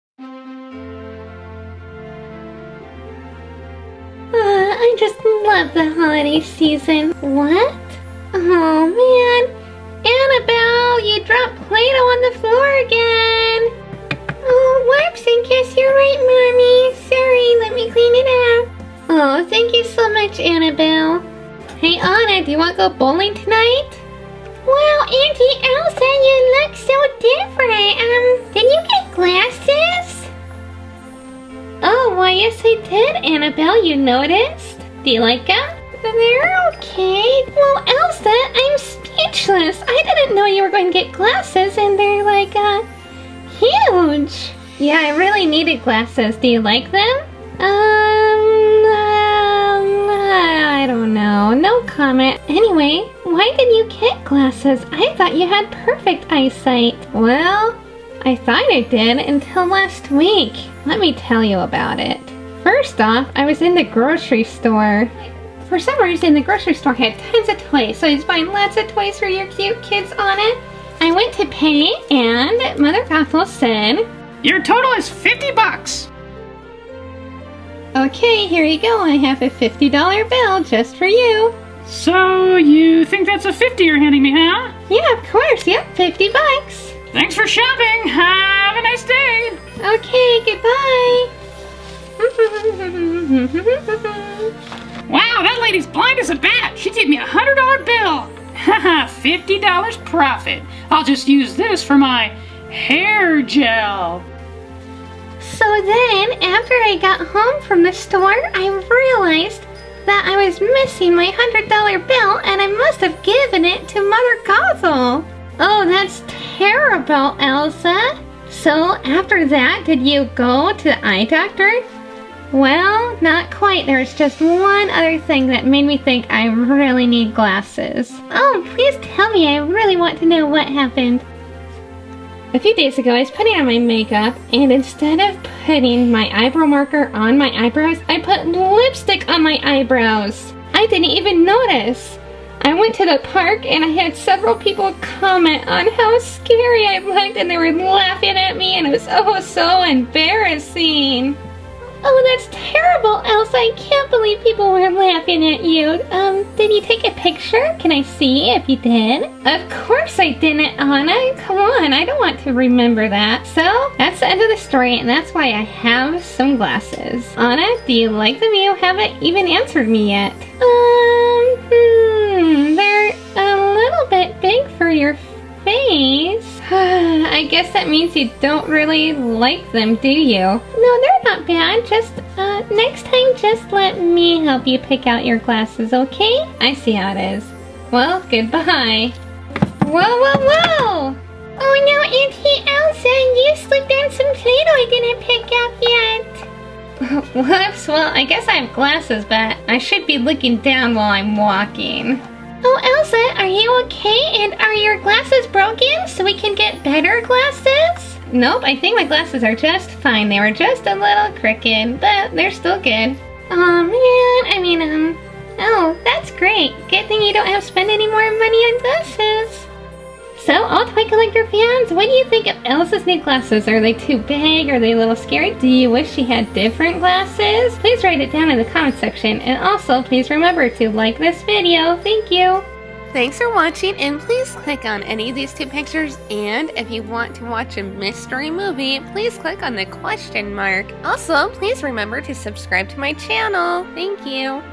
Disney Elsa Frozen Gets GLASSES + Christmas Toys Shopping
Disney Princess Elsa Frozen GETS GLASSES by Toychannel AllToyCollector. Play-Doh Disney Princess Elsa Frozen Parody with Frozen Annabelle and Anna. Disney Princess Queen Elsa is shopping at Mother Gothel's Toy Store from Disney Tangled, and figures out she needs glasses. Elsa also painted her eyebrows with lipstick. Frozen Annabelle has play-doh on the floor of the KidKraft Dollhouse and Elsa trips on it. Disney Princess Anna does not like Elsa's new glasses and hopes they break.